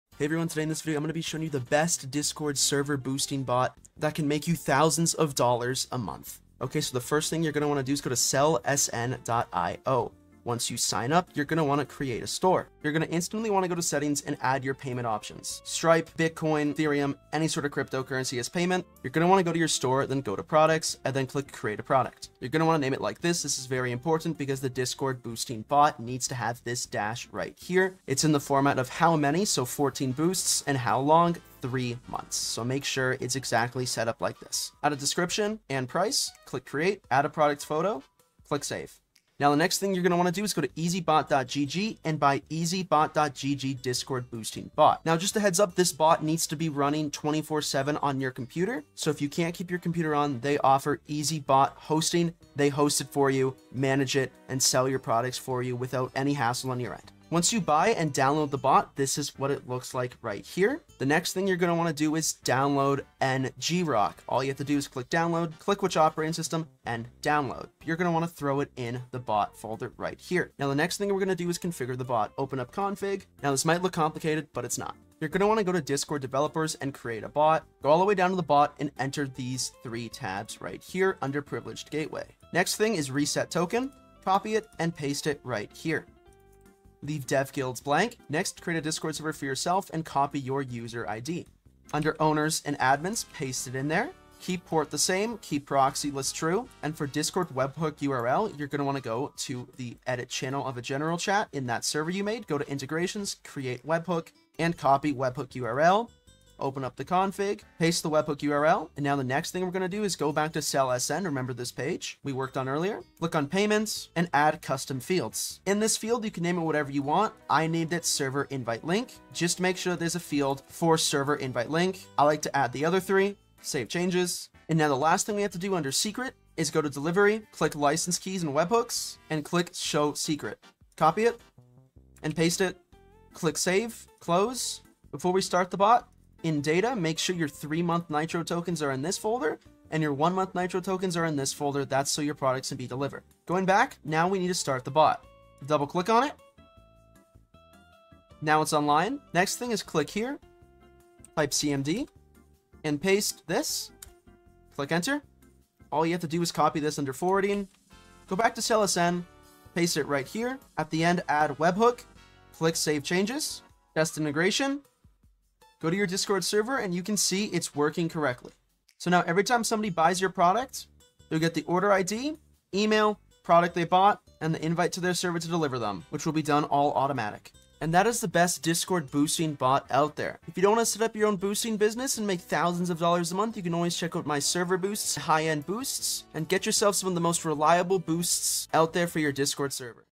BEST DISCORD SERVER BOOSTING BOT (2023)
THIS is the BEST discord SERVER BOOSTING BOT in 2023 and in this video i show you all the steps you have to do to set it up: Download SERVER BOOSTING BOT: ezbot

TO give back to the community, SellSN will be hosting weekly Nitro giveaways, make sure to spread to your friends and grow the community